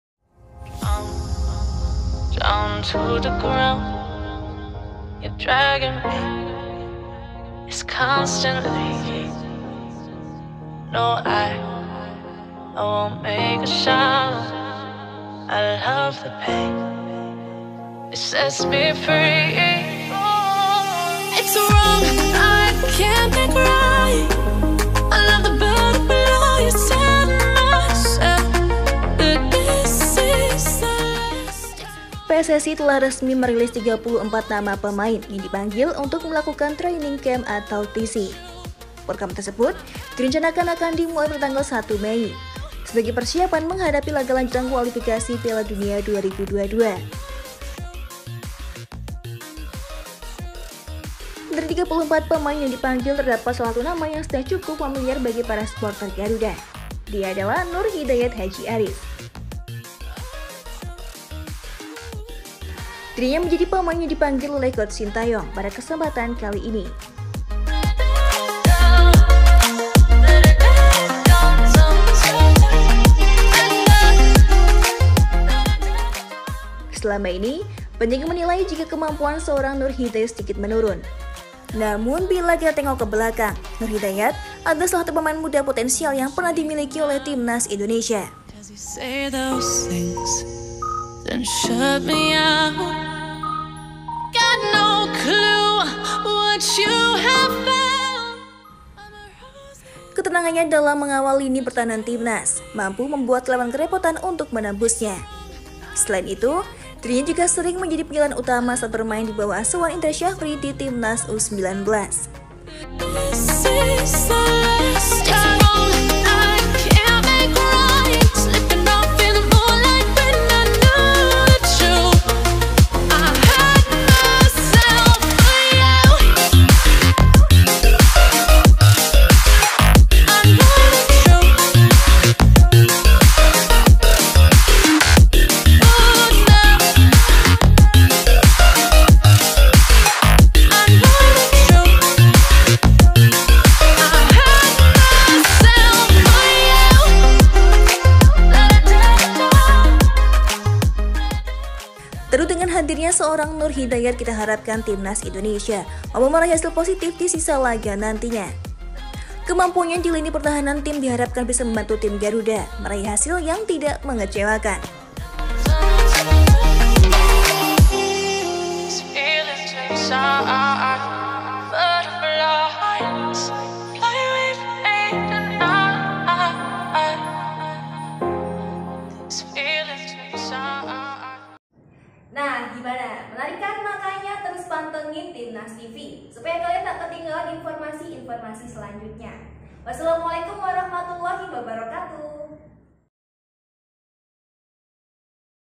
YAKIN PEMAIN TITIPAN ⁉️Lihat Aksi Nurhidayat yg Buktikan Ia Bukan Pemain Titipan di Timnas Indonesia
YAKIN PEMAIN TITIPAN ⁉️ Lihat Aksi Nurhidayat Haji Haris yang Buktikan Ia Bukan Pemain Titipan di Timnas Indonesia